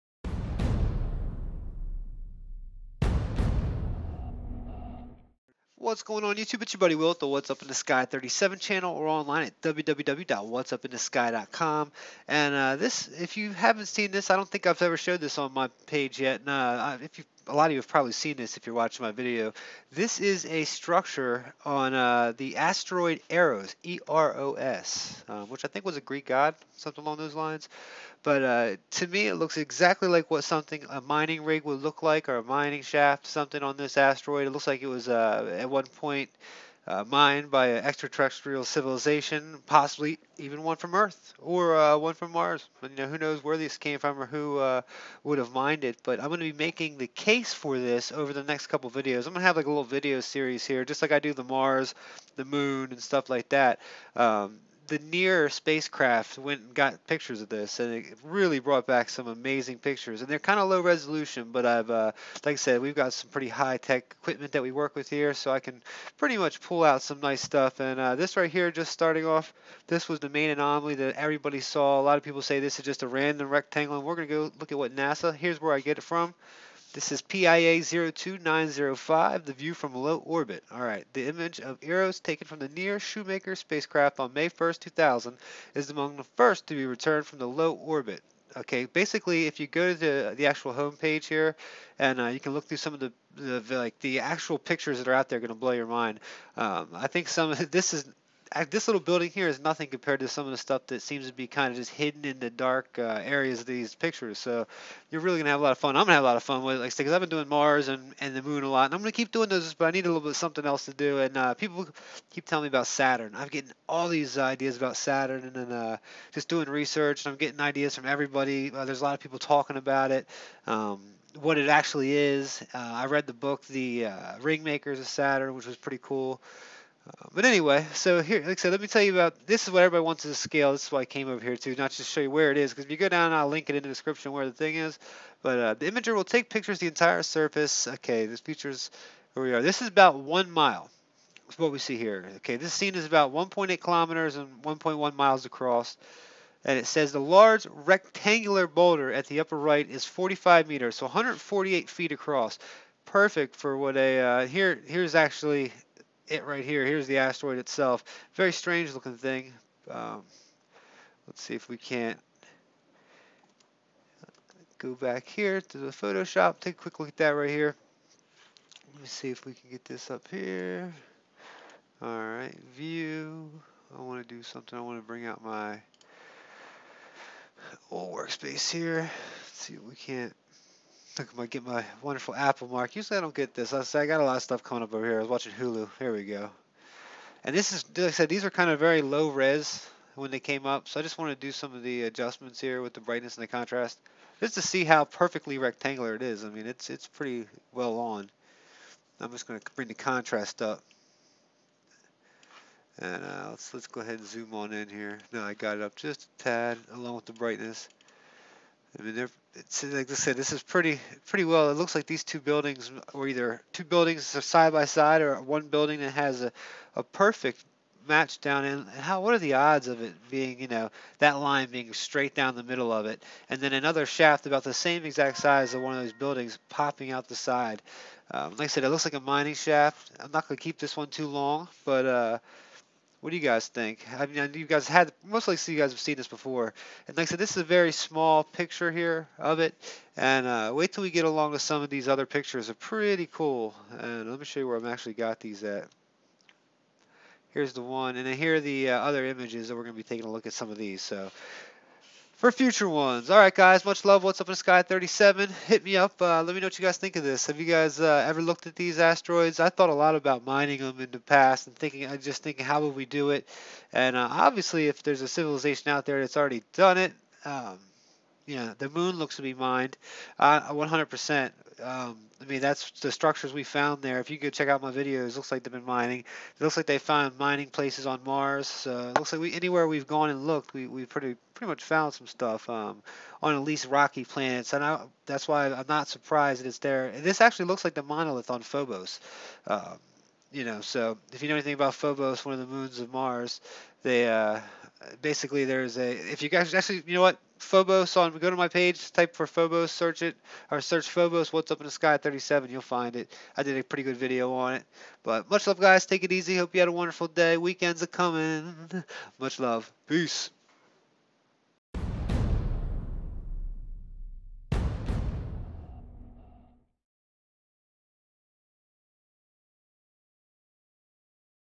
Mining On Asteroid EROS Found By NASA NEAR Spacecraft?
EROS - Video 1: This series will focus on the Asteroid EROS and its amazing features for such a small celestial object.  From the NEAR Spacecraft images I have found many structures and what look to be remnants of an a ancient mining operation on the asteroid.  Much love to you all please let me know what you think! As always I provide you the link..

"I see what I see, You see what you see.." - Me